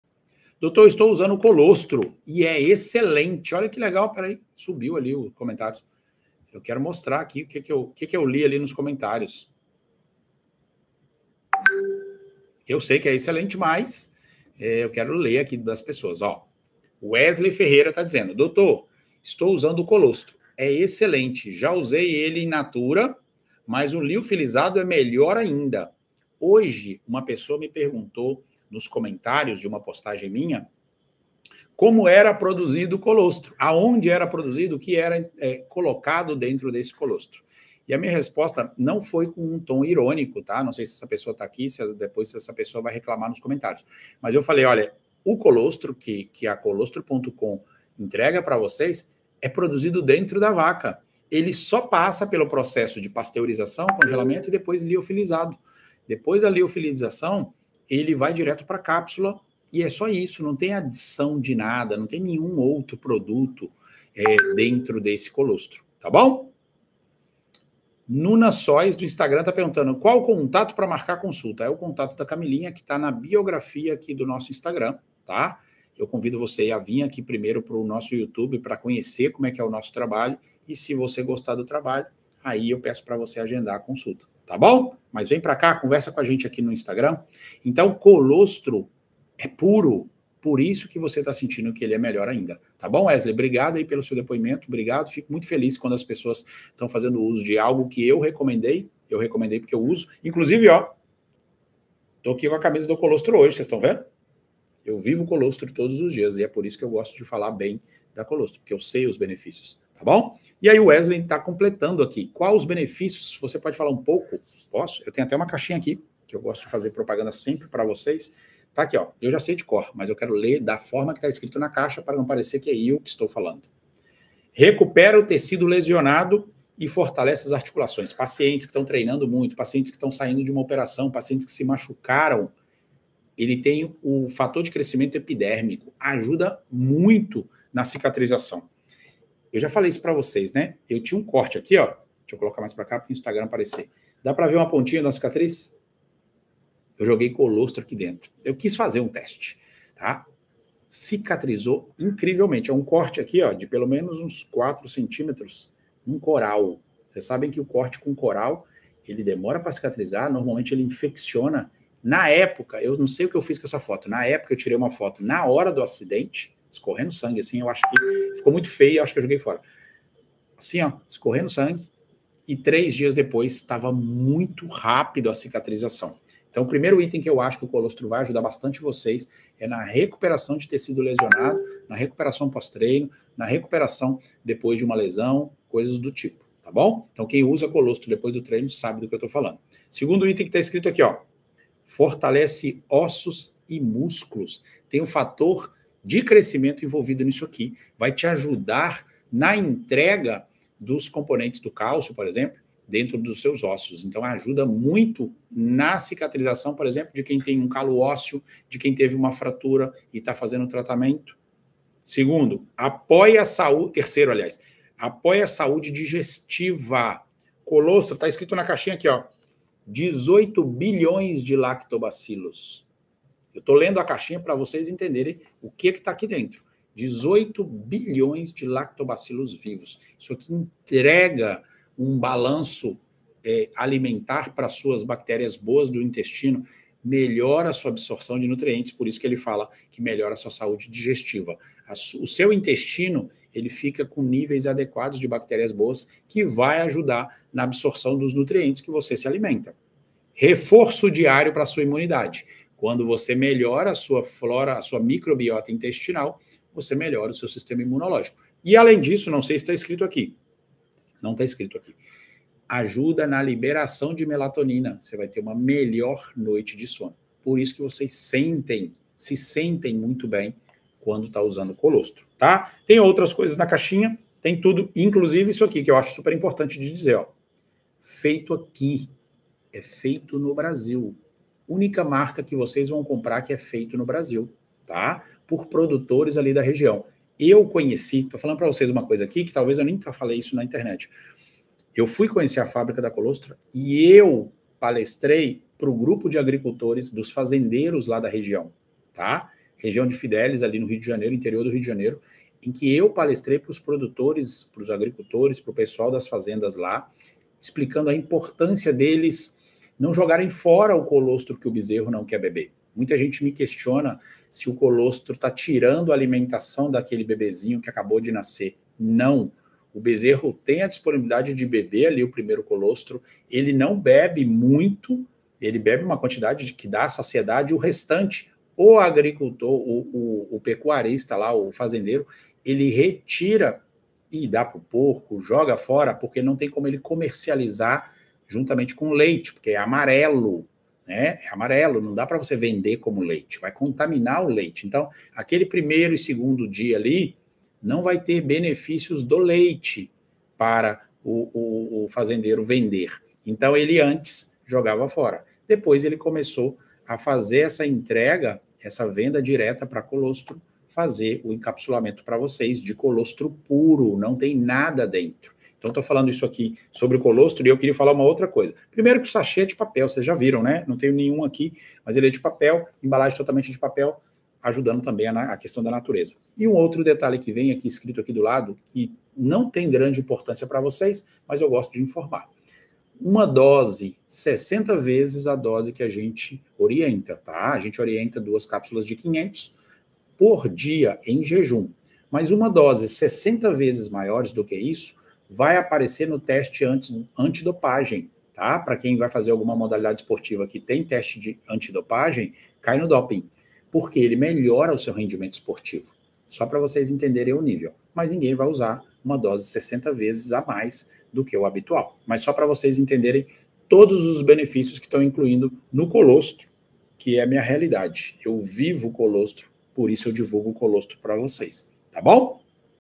COLOSTRO - Benefícios
Corte da live que explica bem sobre o Colostro bovino e seus benefícios.

Ficou interessado?
Acesse e adquiri já o seu.

Use o cupom DOC30.!